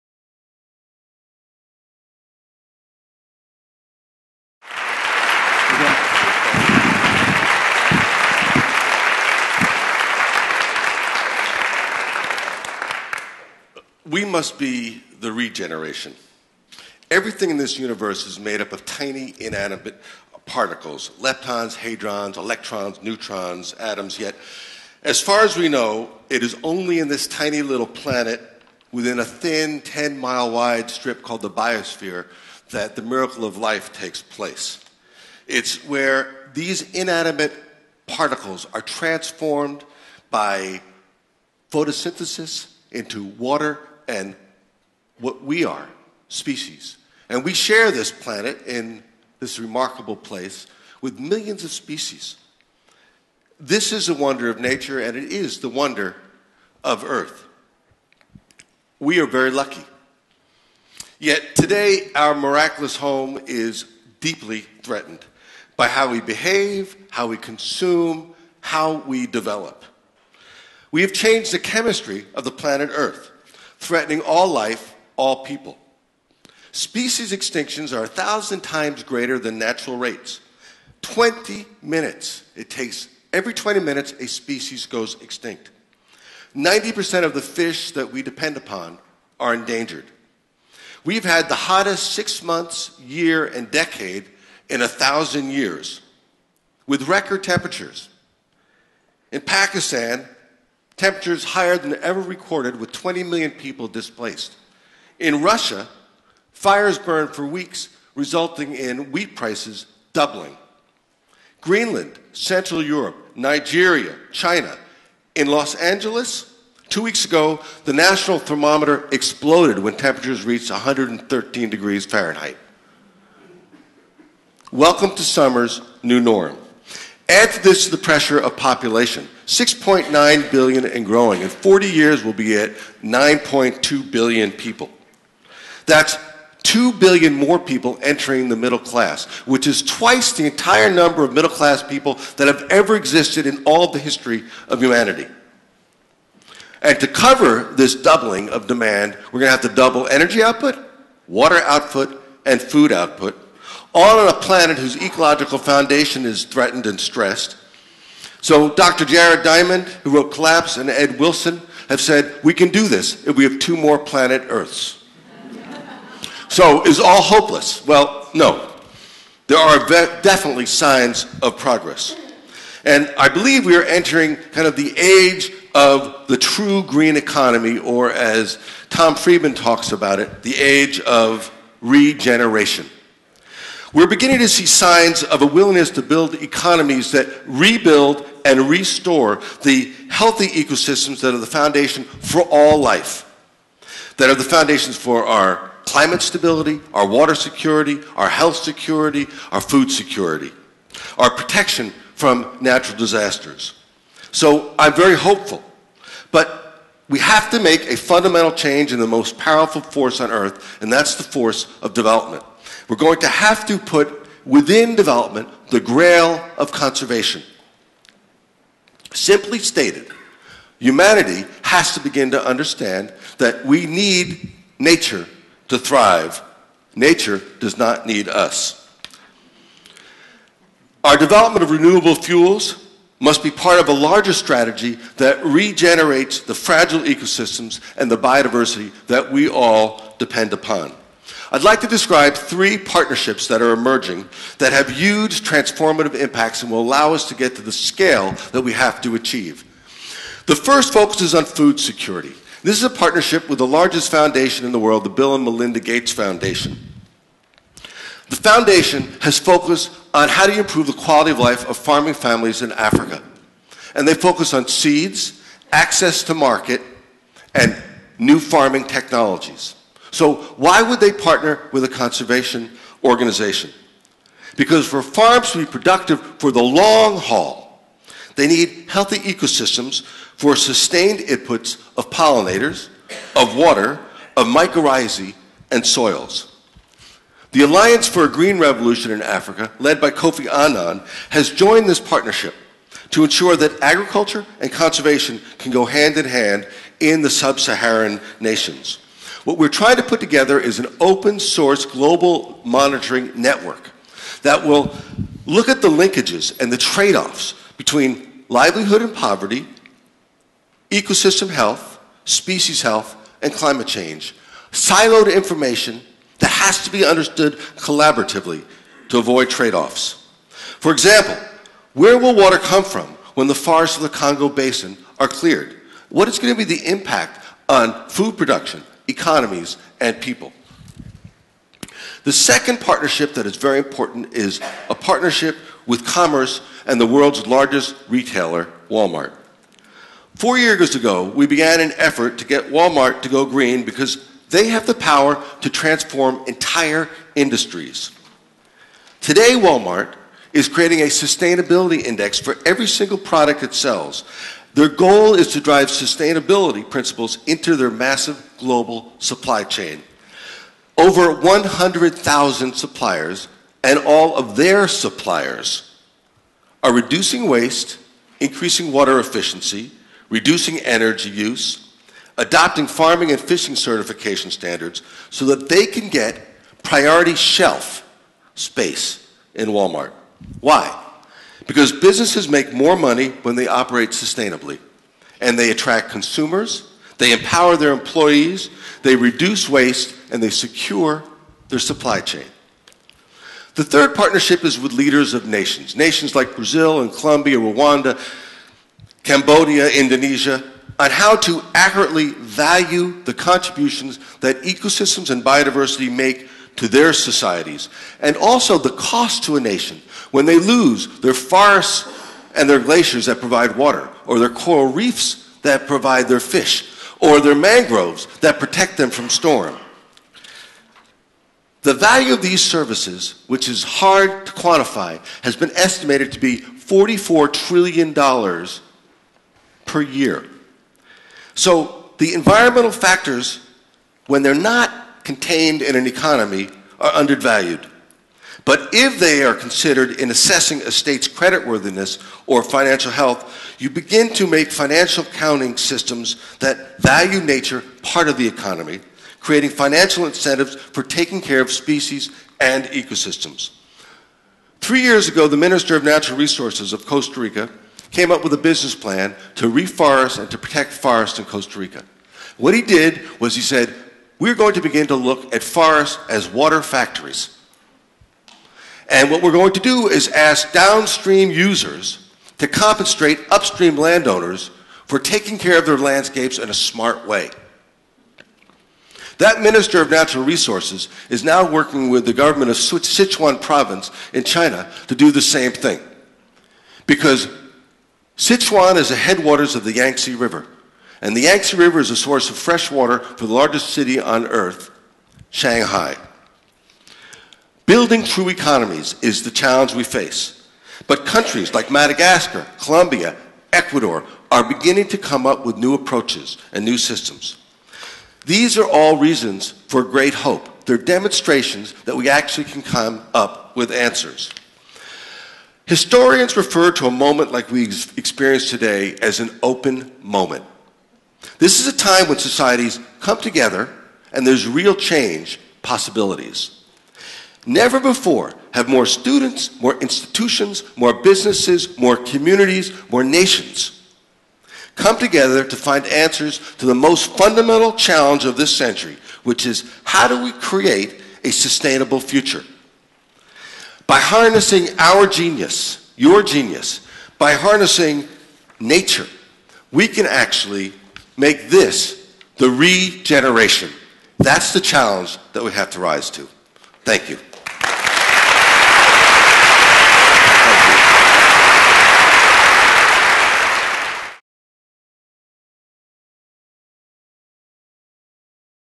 TEDxRainier - Peter Seligmann
Peter Seligmann has a vision and a mission. The vision is an environmentally healthy world that will provide economic opportunities and security for all people. The mission is to bring together industry, government, religious leaders, professors, scientists, local and indigenous people to make that vision a reality. Seligmann's passion is contagious. People worldwide -- from local communities to captains of industry to heads of state -- are buying into Seligmann's bold agenda.

Constantly bringing new ideas and innovation to the table, Seligmann lead Conservation International's growth into one of the foremost forces for conservation today. Widely recognized as one of the world's most inspiring and dynamic environmental entrepreneurs, Seligmann focuses on finding strategic positive tipping points: activities and partners that will allow Conservation International's work to have a truly global impact. He is an authority on how the power of the private sector can be harnessed to protect the Earth's biodiversity. The future Seligmann imagines -- in which our Earth's natural wealth serves as the cornerstone for vibrant, thriving human societies -- is attainable, but it will require a global shift in attitudes and actions, a shift to ensure that generations to come can enjoy Earth's abundant biodiversity.

TEDxRainier is an independently organized TED event held in Seattle, Washington.